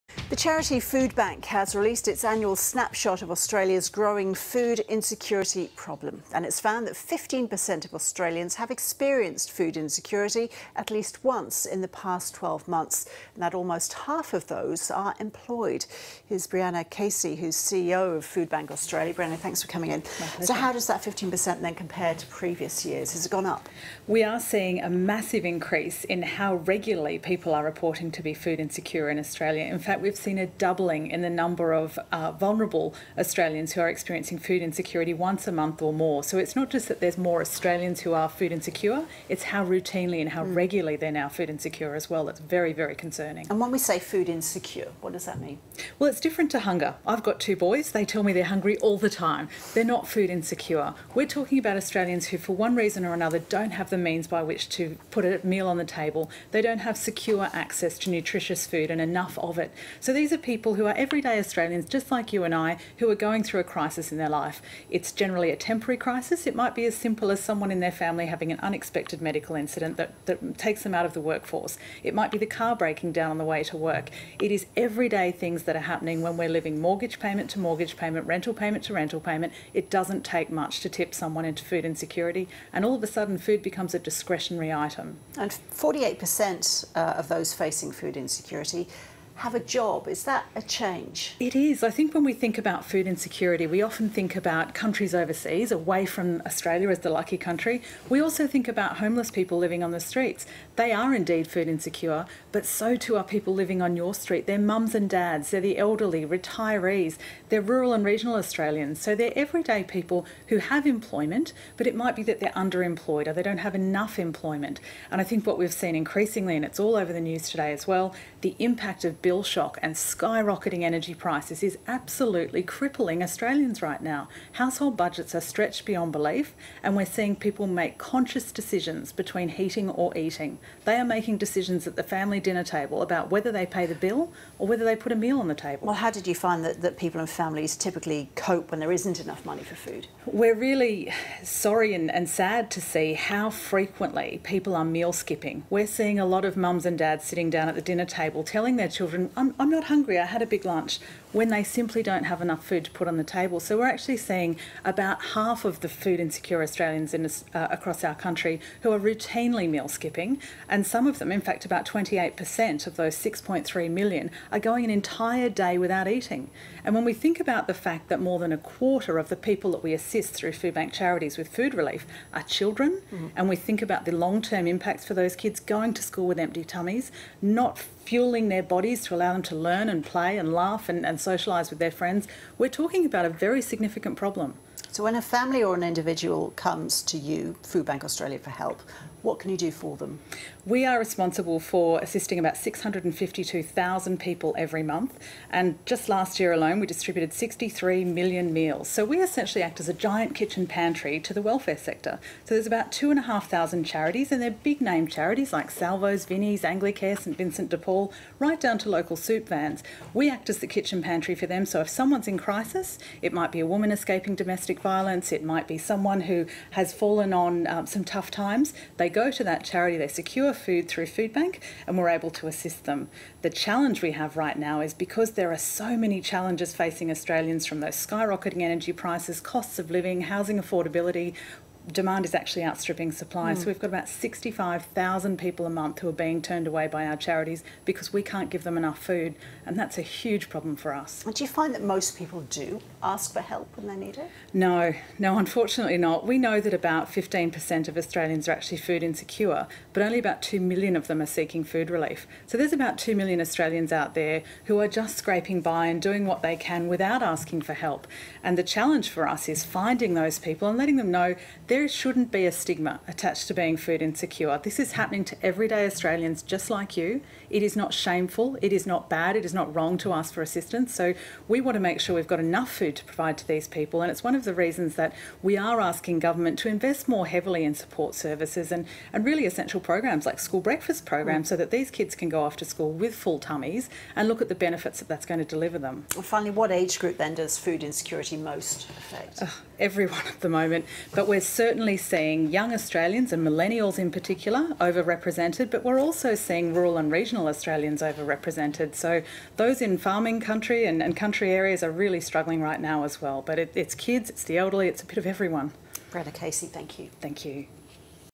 Foodbank Hunger Report 2017 - ABC News24
On World Food Day yesterday, we released our annual Foodbank Hunger Report for 2017.
This year's results are staggering, to say the least, with 3.6 million Australians experiencing food insecurity in the last 12 months - that's 15% of the population.
The report found that the rising cost of living has severely impacted the choices people make when it comes to buying more food or paying their bills, with 56% saying that large or unexpected bills are the reason they are unable to purchase food.